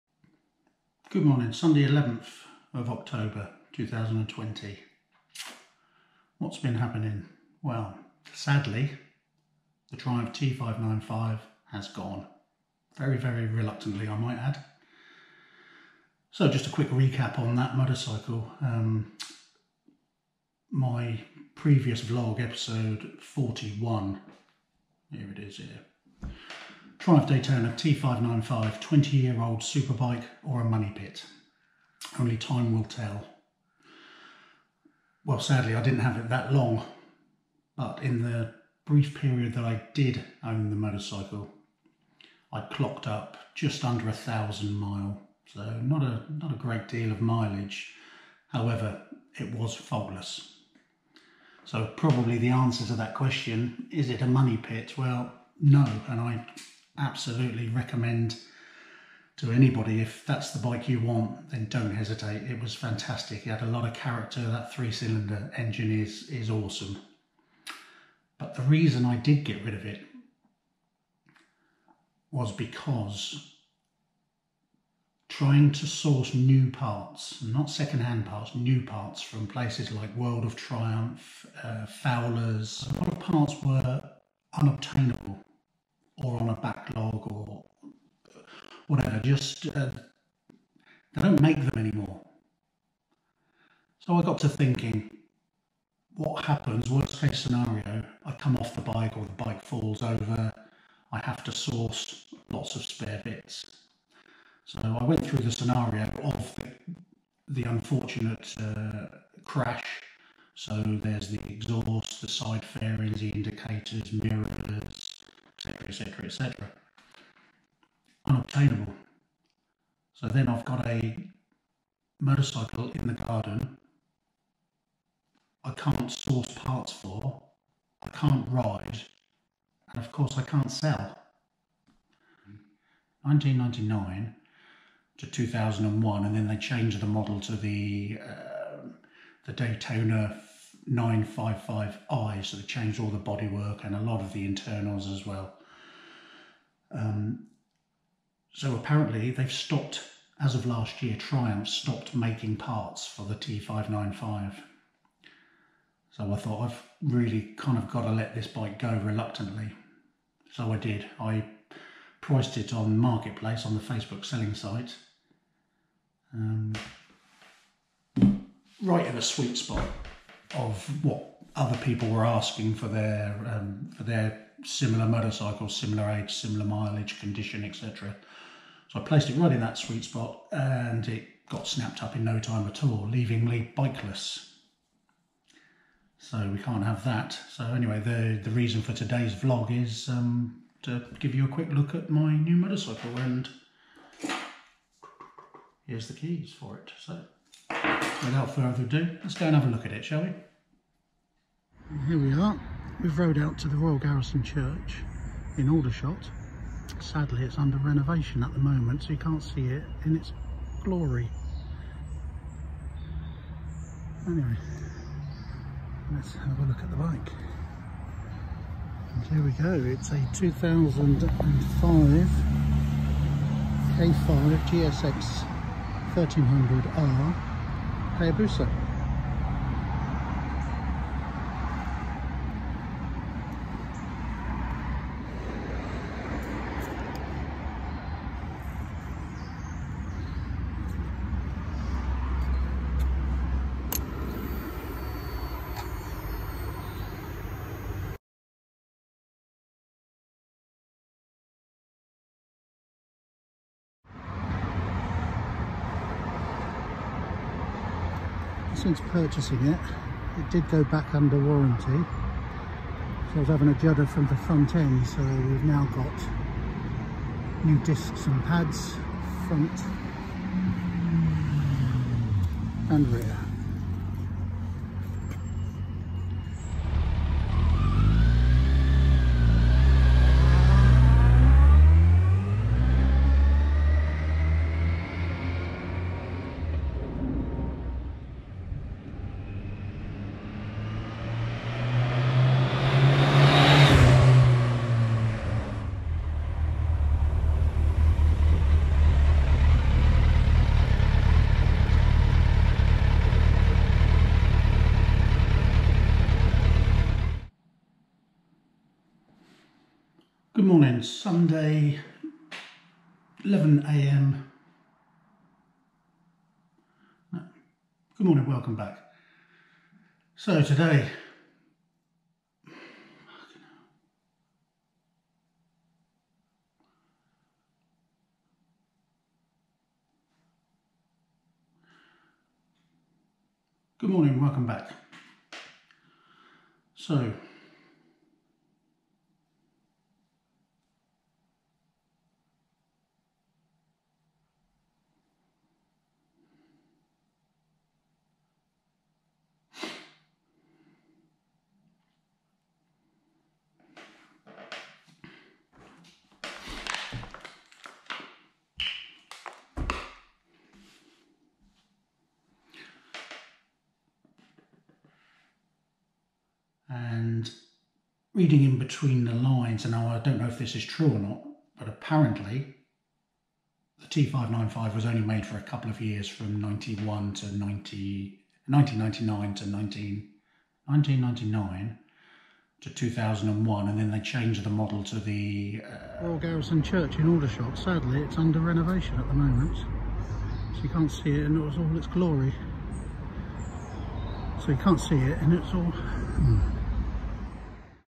Ep 44 Triumph Daytona T595 : Sadly sold.But what is it’s replacement? + Take Twos
Having reluctantly let the Triumph go to it’s new owner, I opt for the mighty Hayabusa. Not everyone’s cup of tea, love or hate them, they are undeniably fast. An initial front end chatter was cured by new discs and pads.
The plan is to deep clean, replace anything that needs attention, like corroded fasteners and disc bolts. In the winter, send the brake calipers off to be refurbished back to factory and basically make this ‘Busa as good as it can be. That’s the plan!